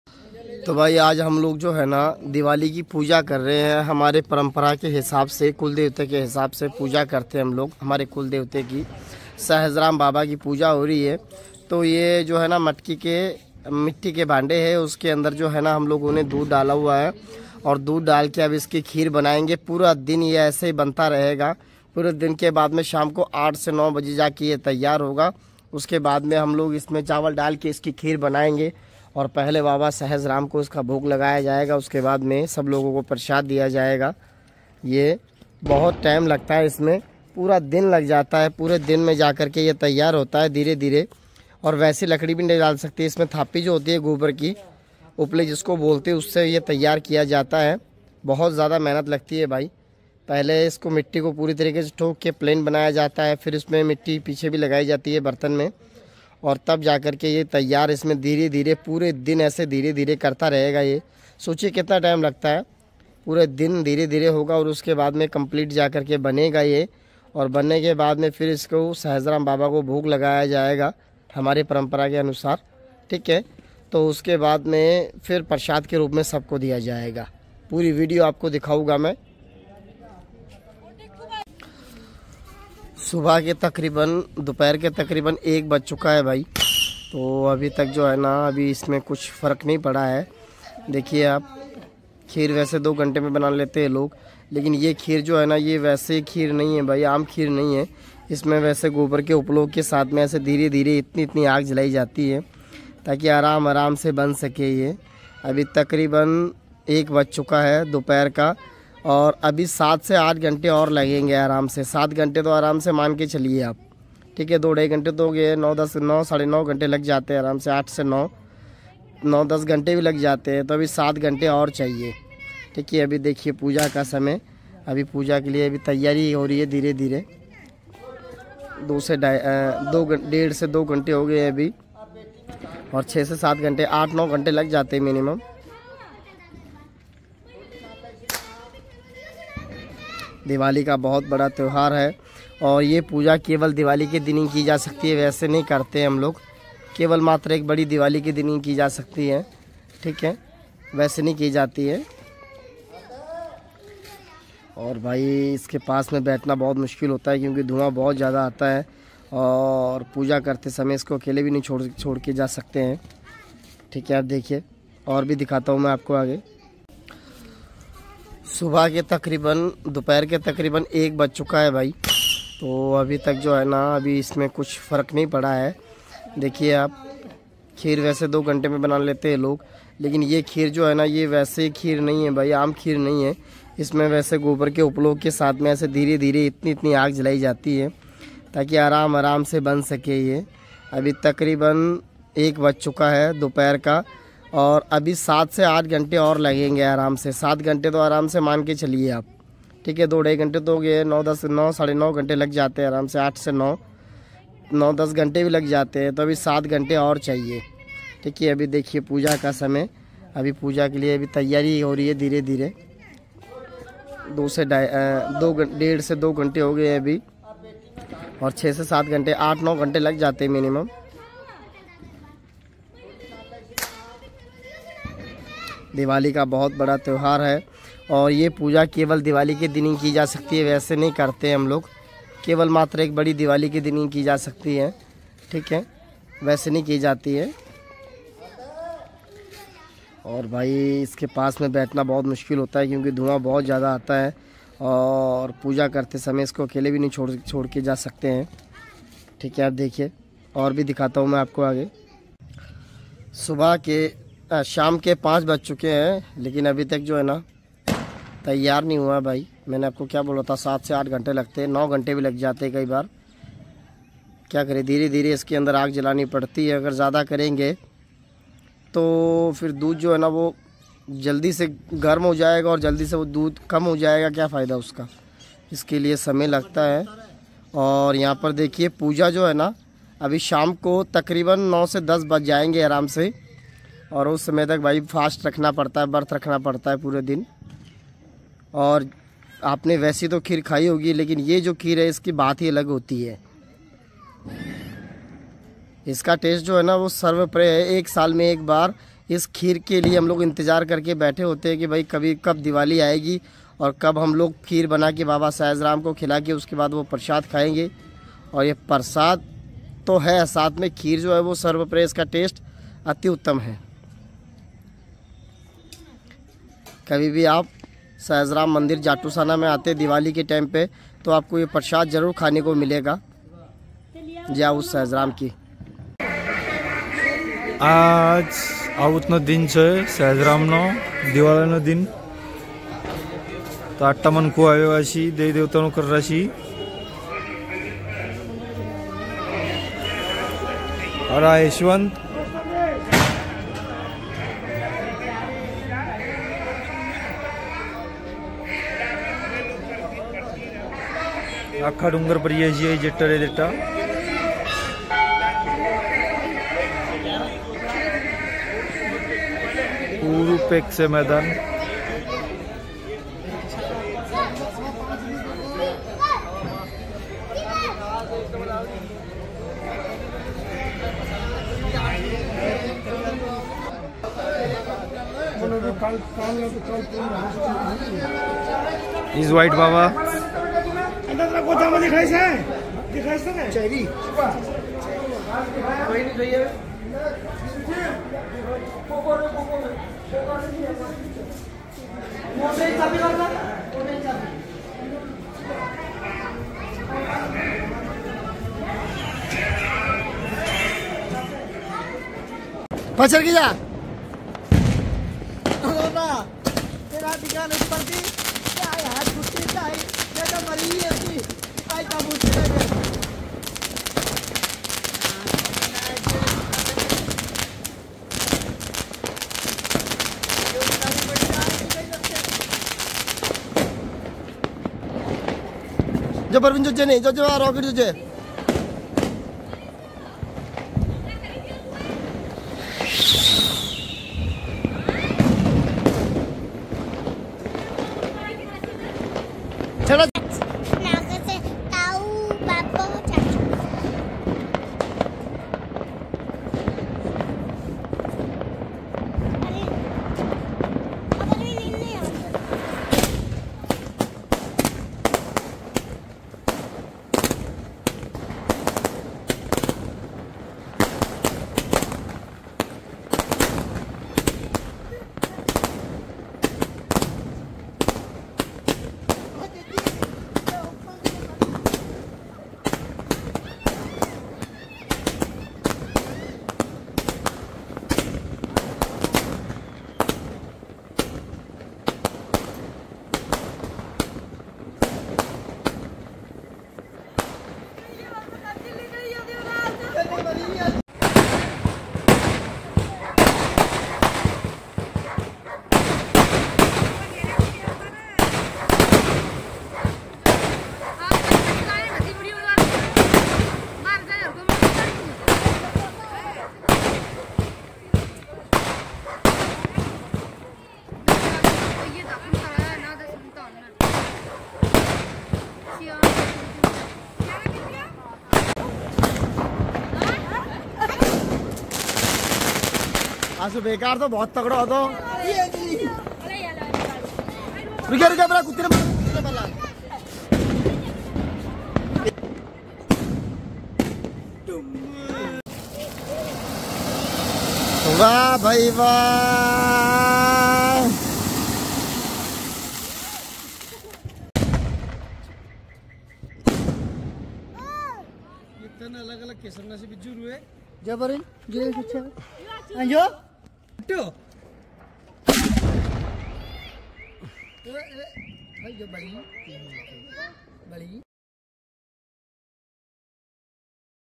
13 November 2023
दिवाली पर बाबा सहज राम को खिलाया जाता है यह प्रसाद है पूरे दिन में तैयार होता है यह प्रसाद🛕🛕🛕🛕🛕🛕🛕🛕🛕🛕 इस वीडियो को पूरा जरूर देखें और कमेंट में जय श्री राम लिखें💐💐💐💐💐💐🪷🪷🪷🪷🕉🕉🕉🕉🕉🕉🕉